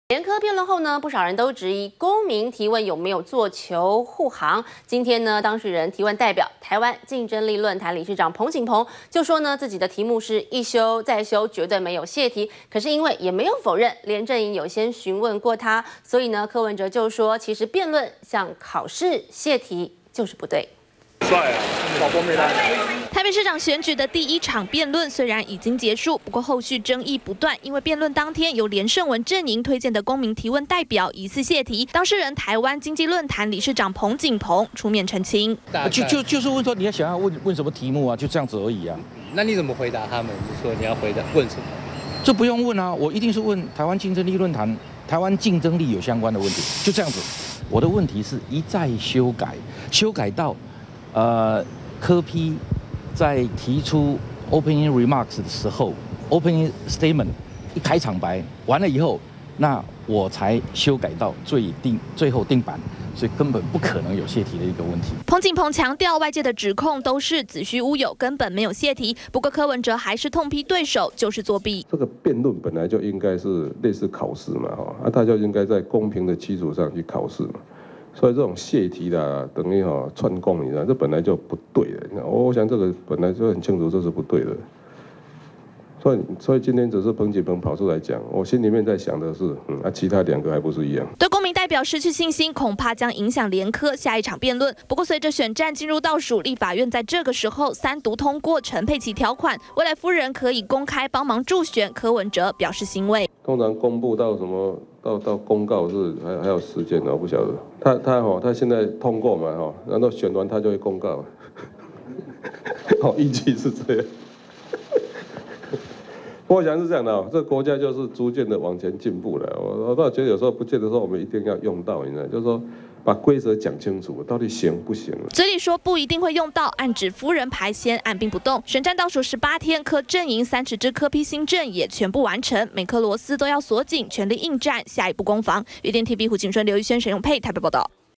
【2014.11.11】坦承連營打探題目 彭錦鵬駁洩題 -udn tv
連柯辯論後，不少人都質疑公民提問，有沒有做球護航?今天當事人提問代表，台灣競爭力論壇理事長彭錦鵬就說，自己題目一修再修，絕對沒有洩題。但因為也沒否認連陣營，有先詢問過他。完整內容 , FB , Twitter , G+